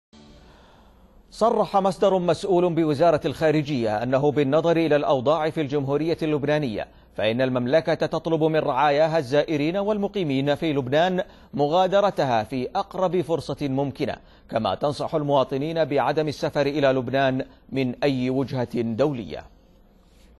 السعودية تطلب من رعاياها مغادرة لبنان فورا
نقلت وكالة الأنباء السعودية عن مصدر مسؤول بوزارة الخارجية السعودية قوله إن المملكة طلبت من رعاياها بلبنان مغادرة البلاد في أقرب وقت ممكن.

ونسبت الوكالة إلى المصدر قوله "بالنظر إلى الأوضاع في الجمهورية اللبنانية فإن المملكة تطلب من رعاياها الزائرين والمقيمين في لبنان مغادرتها في أقرب فرصة ممكنة، كما تنصح المواطنين بعدم السفر إلى لبنان من أي وجهة دولية."

عاجل_واسمصدر مسؤول بوزارة الخارجية : المملكة تطلب من رعاياها الزائرين والمقيمين في لبنان مغادرتها في أقرب فرصة ممكنة كما تنصح المواطنين بعدم …